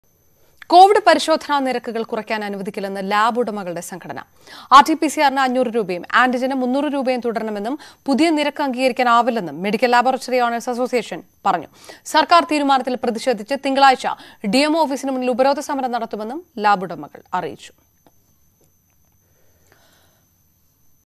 Covid പരിശോധനാ നിരക്കുകൾ കുറയ്ക്കാൻ അനുവദിക്കില്ല; പ്രതിഷേധത്തിനൊരുങ്ങി ലാബുടമകൾ
Covid പരിശോധനാ നിരക്കുകൾ കുറയ്ക്കാൻ അനുവദിക്കില്ലെന്ന് ലാബുടമകളുടെ സംഘടന. സർക്കാരിന്റെ പുതിയ നിരക്ക് അനുവദിക്കാനാകില്ലെന്നും സർക്കാർ തീരുമാനത്തിൽ പ്രധിഷേധിക്കുമെന്നും ലാബുടമകൾ വ്യക്തമാക്കി.